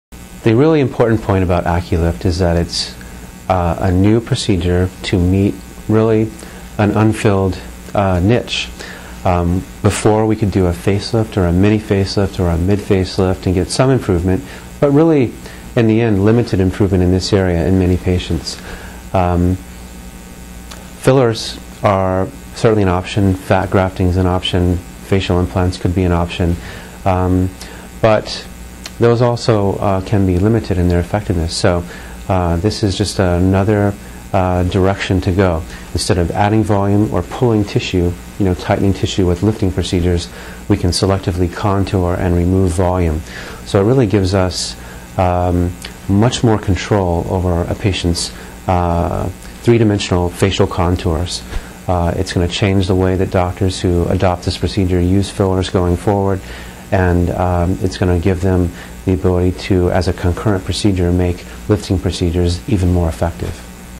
Dr Holcomb interview about AccuSculpt - New facial lifting technology
"This laser is not some copy of an existing technology. This is a novel treatment wavelength based on sound science."
- Dr Holcomb -

Cleared by the FDA, the AccuSculpt is effective at achieving desired contours while gently sculpting the face and body, without the side effects of other lipo procedures. It is safe for all areas of the body including abdomen, face, neck, inner thighs, flanks, buttocks, knees, back and arms.